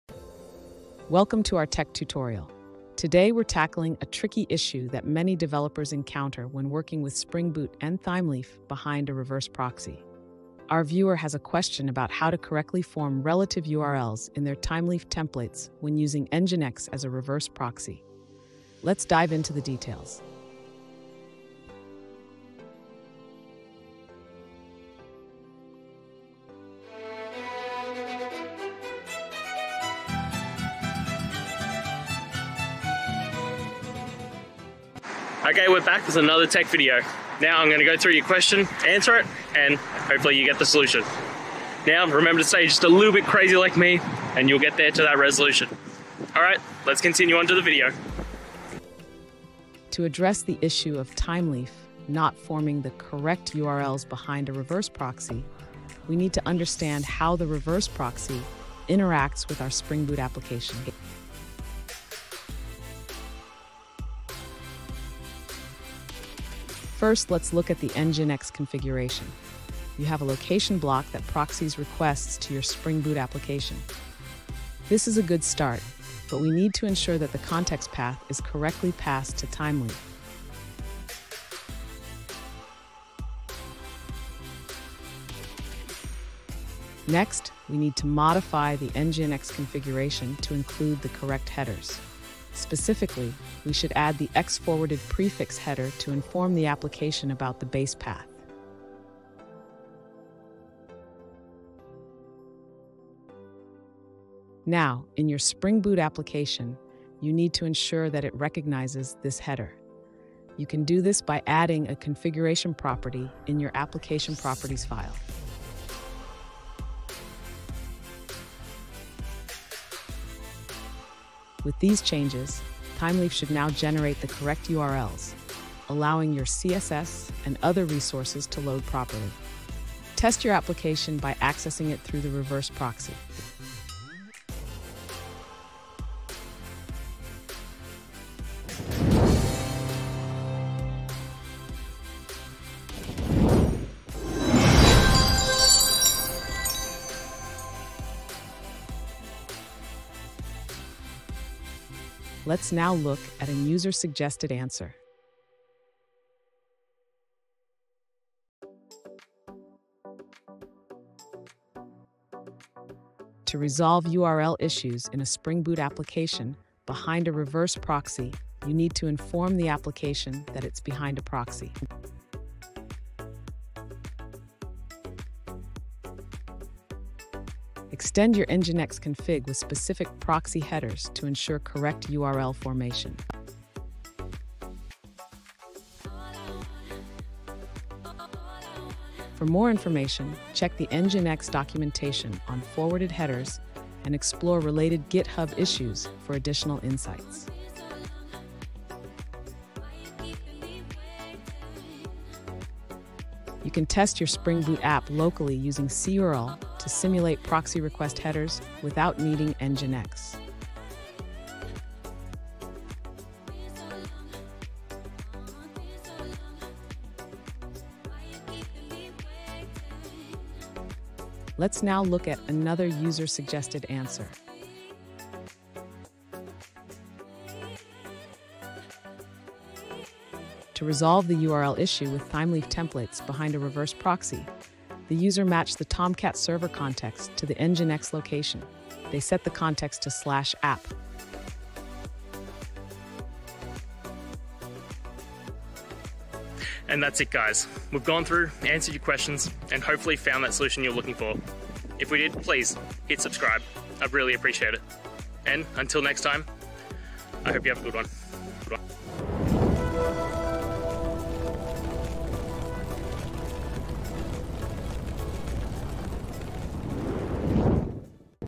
Fixing Thymeleaf URL Issues in Spring Boot Behind a Reverse Proxy
In this video, we delve into the common challenges developers face when using Thymeleaf with Spring Boot applications deployed behind a reverse proxy. We'll explore how reverse proxies can affect URL handling and rendering, leading to unexpected behavior in your web applications. Join us as we provide practical solutions and best practices to ensure seamless integration and optimal performance, so you can focus on building great features without the hassle of URL issues.

Today's Topic: Fixing Thymeleaf URL Issues in Spring Boot Behind a Reverse Proxy

Related to